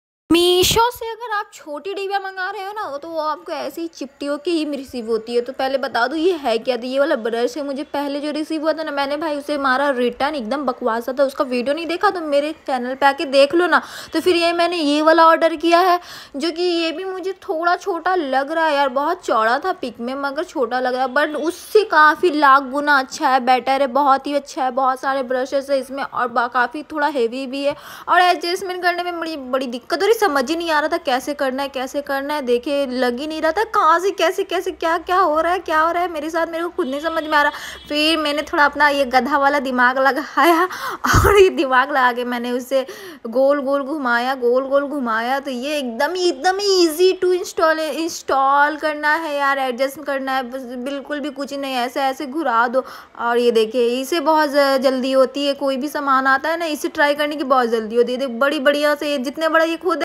meeshofind bathroom cleaning brush 2in1 worth it ✅
Stainless Steel Floor Scrub Brush With Long Handle -50" Stiff Brush, 2 In 1 Scrape And Brush,Tub And Tile Brush For Cleaning Bathroom, Patio, Kitchen, Wall And Deck